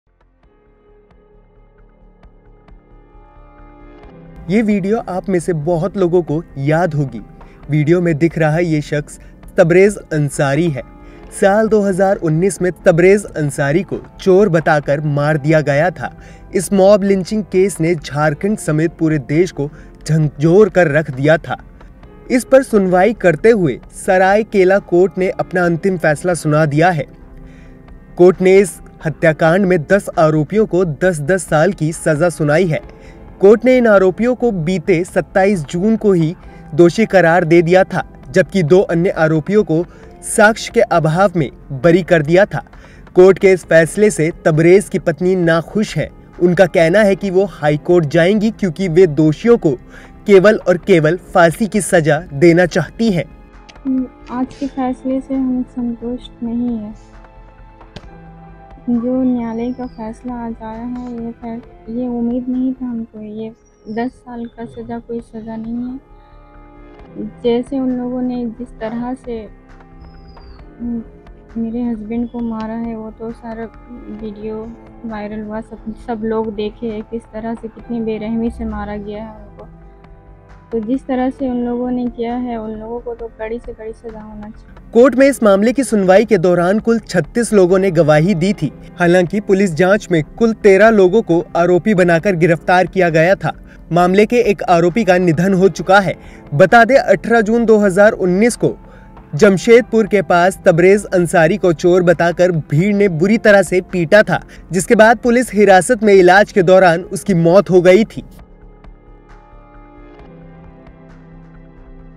Tabrez Ansari murder case में पत्नी ने मांगी फांसी की सज़ा | दैनिक भास्कर हिंदी
ये वीडियो आप में से बुहत लोगों को याद होगी... वीडियो में दिख रहा ये शख्स तबरेज अंसारी है... साल 2019 में तबरेज को चोर बताकर मार दिया गया था... इस मॉब लिचिंग केस ने झारखंड समेत पूरे देश को झकझोर दिया था... इस पर सुनवाई करते हुए सरायकेला कोर्ट अपना अंतिम फैसला सुना दिया है... कोर्ट ने  इस हत्याकांड में 10 आरोपियों को 10-10 साल की सज़ा सुनाई है...